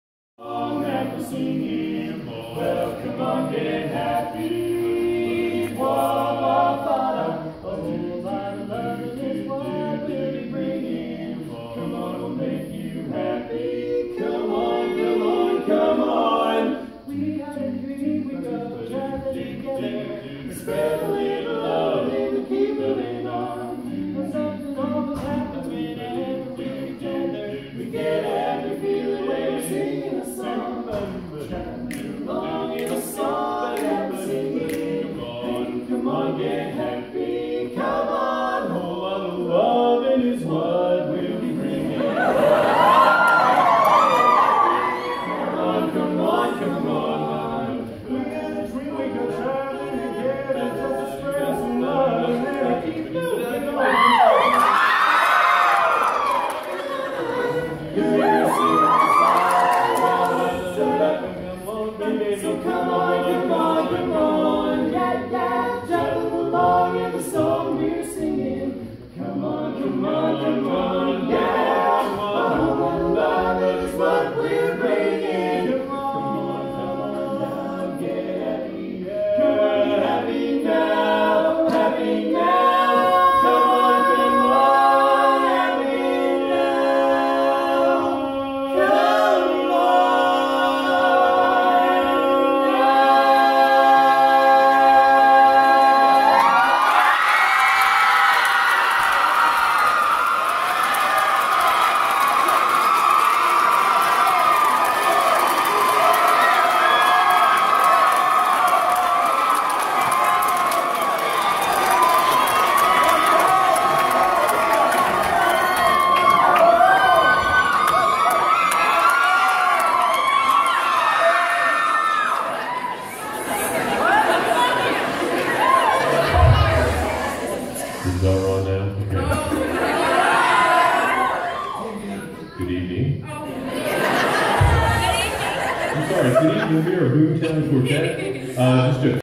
Boomtown quartet at Holstein
Not a great angle, but does capture some of the excitement from the youth in harmony clinic at Holstein.